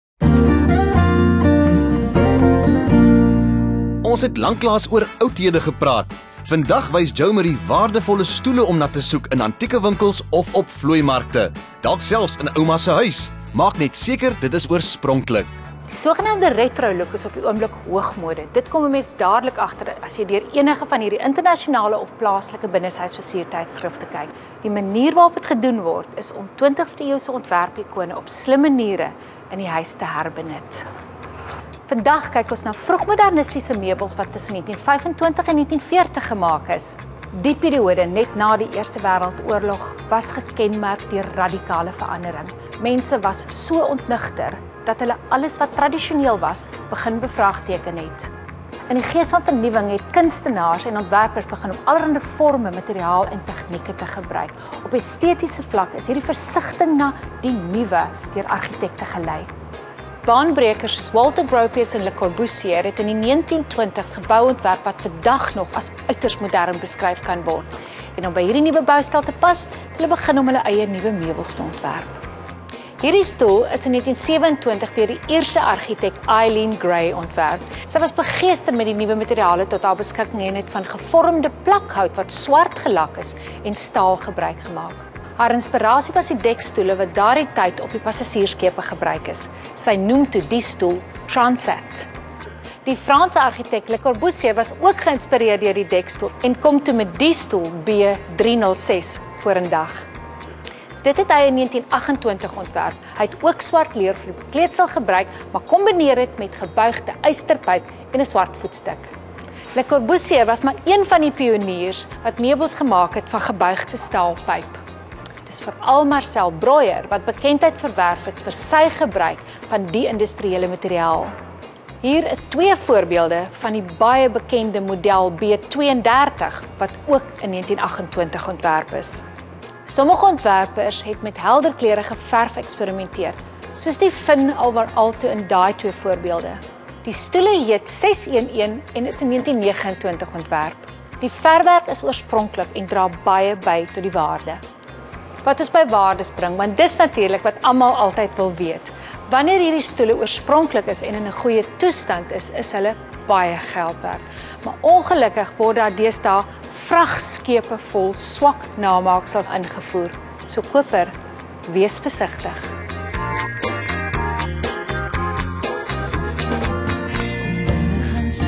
WATER: Jo-Marie Rabé gesels oor retro-meubels
Hoe weet jy watter retro-meubels regtig hul prys werd is?
» Jo-Marie Rabé gesels oor retro-meubels